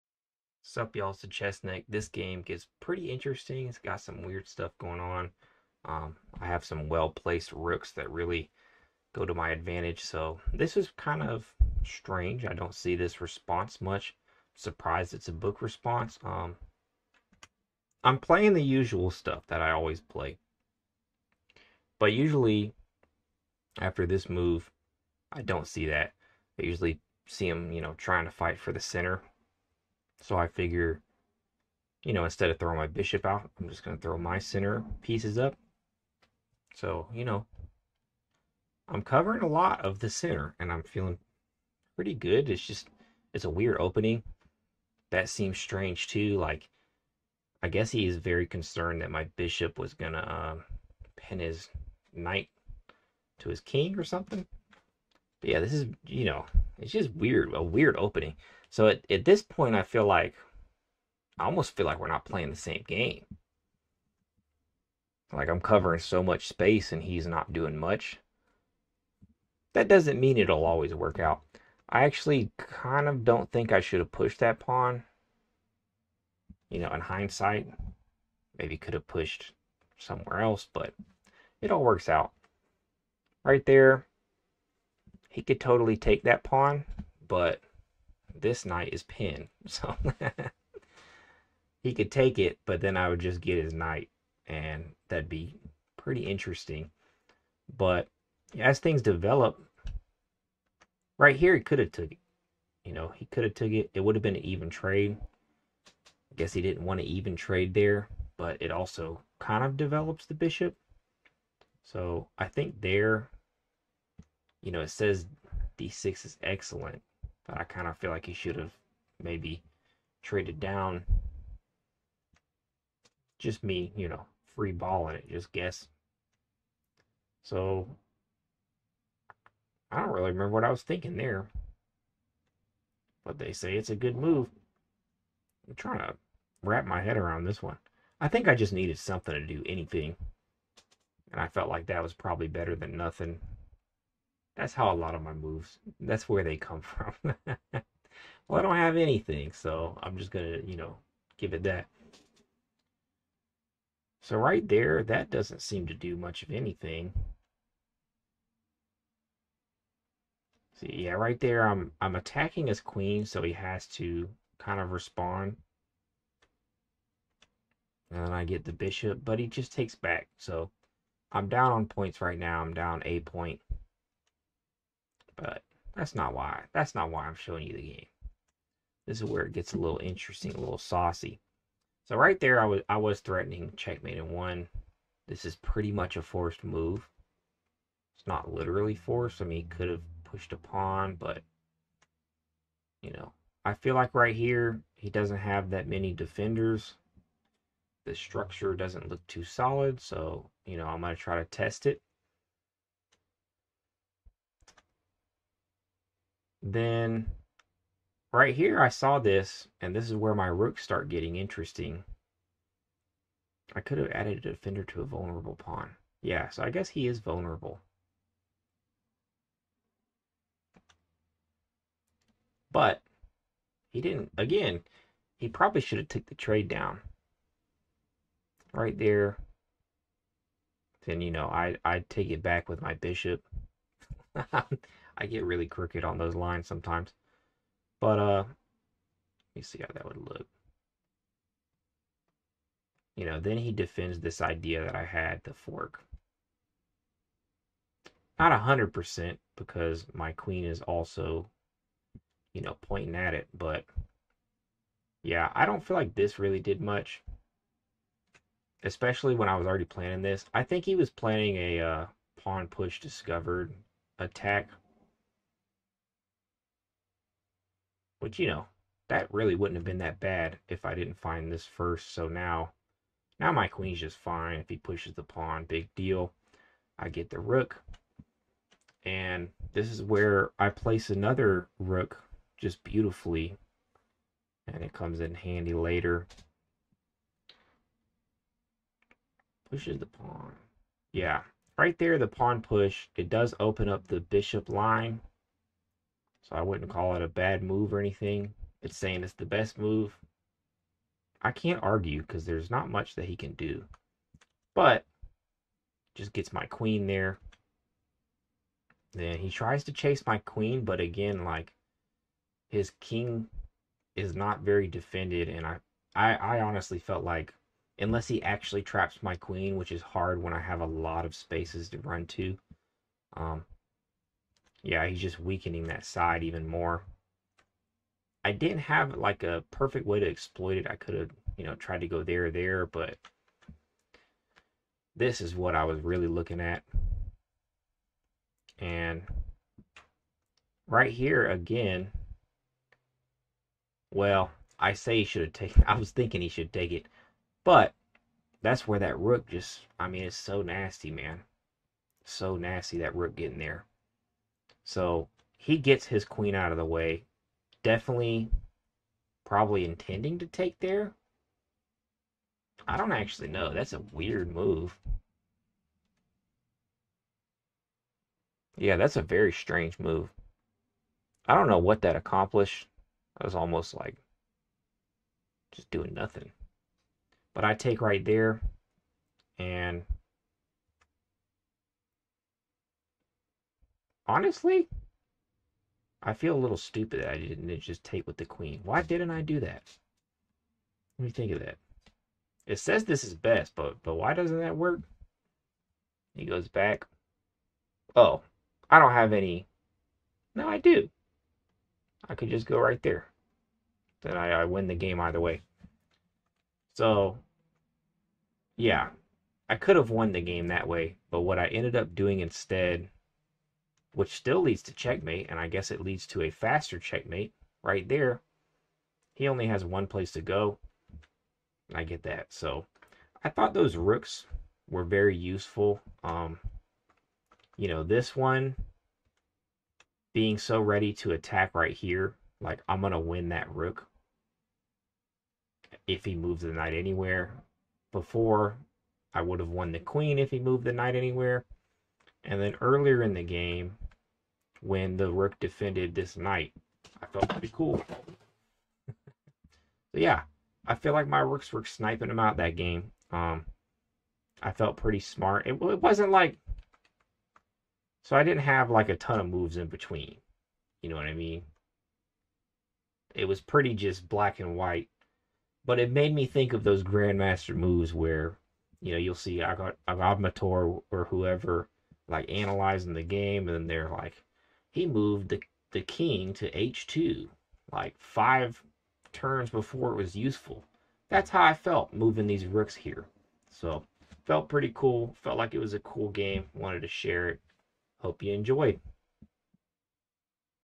Rooks were hitting bullseyes this game
The point of my channel is exactly as described, I play chess!!

I try to keep it as entertaining as possible with my commentary, but at the end of the day I know only giant chess fans will be interested.